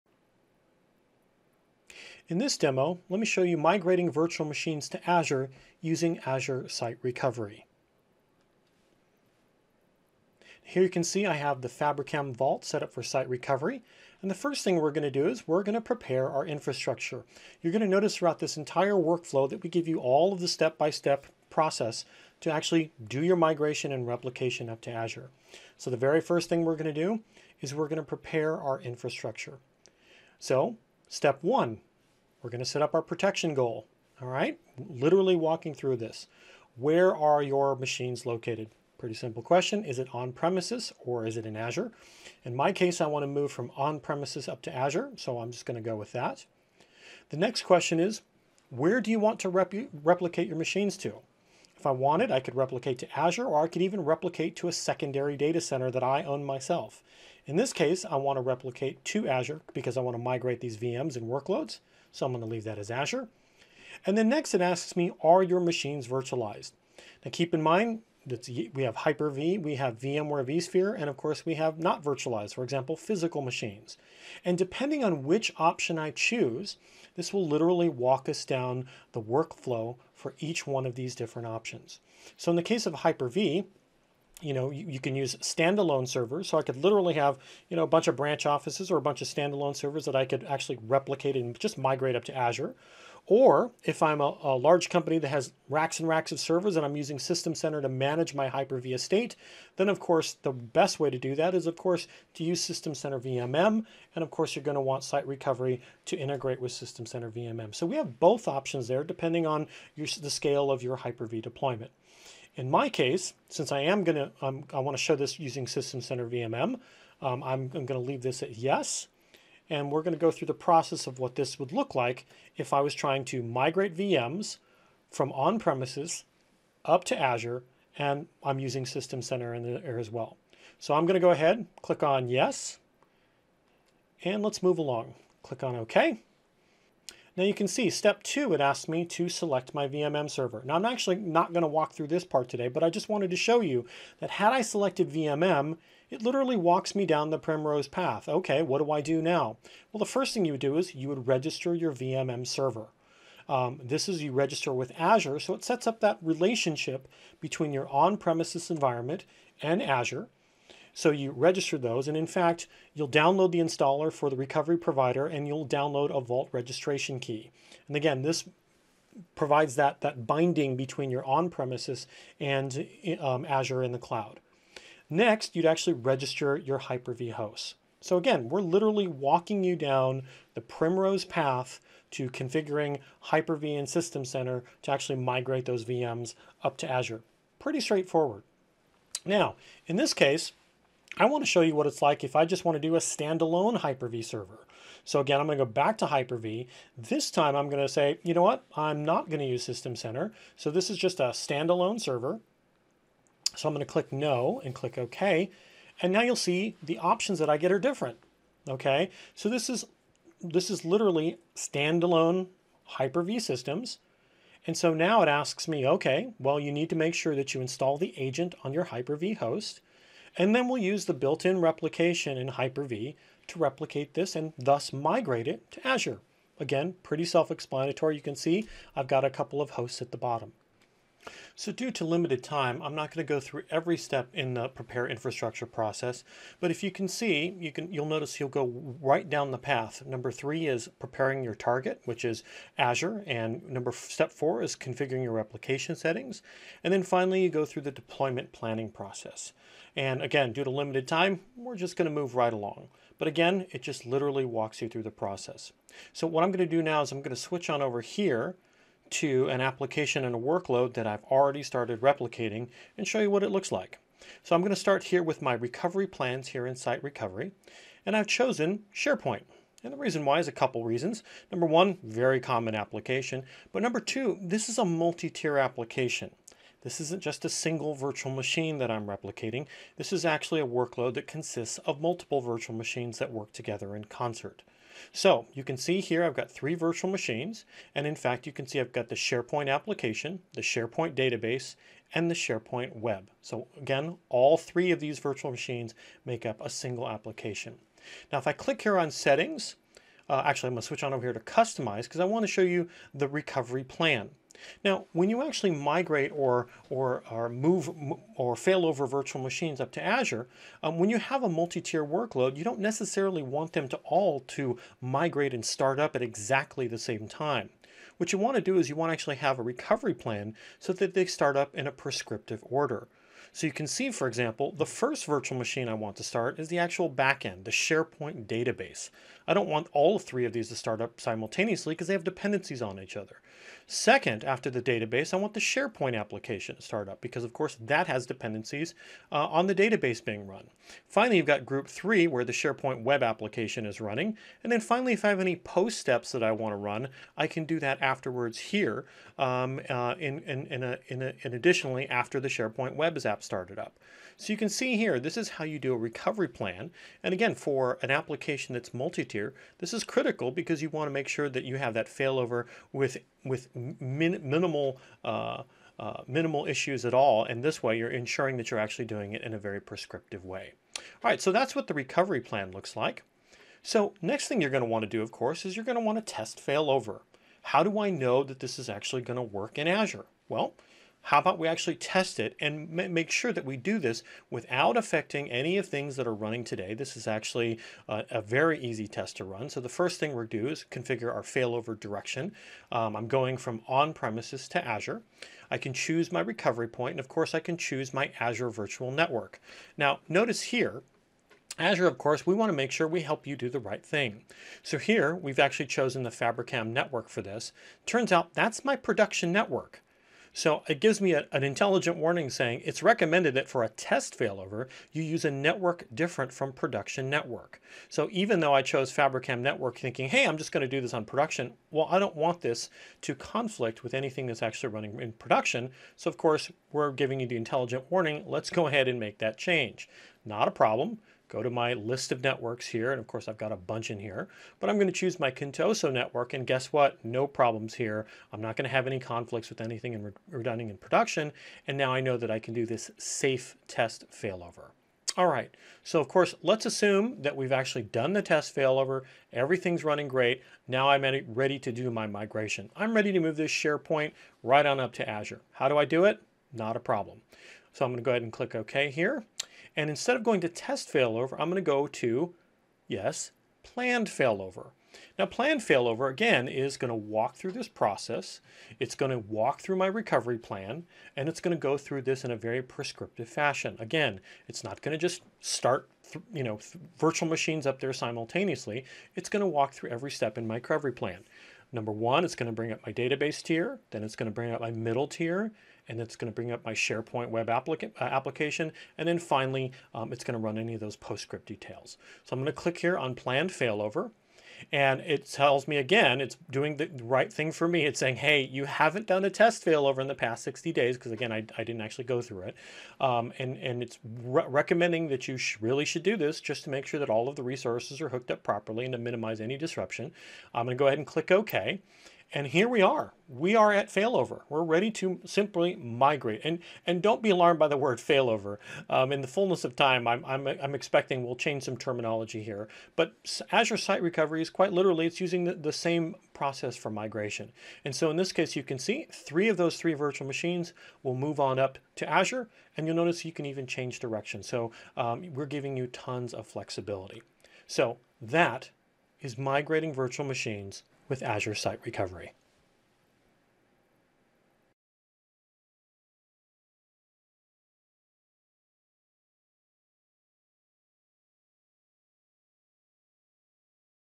How to migrate VMs to Azure using Azure Site Recovery
Find out how easy it is to migrate virtual machines to Azure using Azure Site Recovery, which is a great first step to adding Azure to Windows Server workloads. Microsoft’s Jeff Woolsey will walk through the process of migrating multiple VMs managed by System Center from on-premises to Azure. We also see the process for replicating a standalone server to Azure.